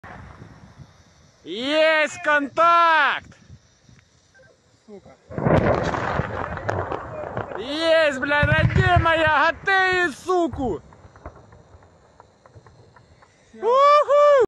MOMENT the German-built and supplied Gepard air defence system shooting down a Russian projectile
A video released by the Ukrainian Air Force on Thursday purported to show the German-built and supplied Gepard air defence system shooting down a Russian projectile.

Ukraine came under heavy Russian bombardment on Tuesday but according to authorities, 60 of the 70 missiles fired were shot down by various kinds of air defence systems.

Three people were nevertheless killed in the shellings on that day, according to Ukrainian President Volodymyr Zelenskyy.

Germany supplied Ukraine with some of its air defence systems earlier in the summer and has vowed to deliver more after coming under heavy criticism for appearing apprehensive with aiding the embattled country with weapons.